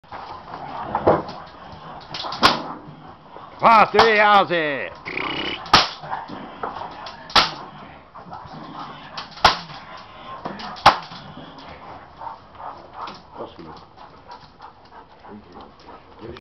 ROMO POLISH BORDER POLICE BREED
Romo a 13 months old Border Police breed . Training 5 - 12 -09 .
Thanks Johan and friends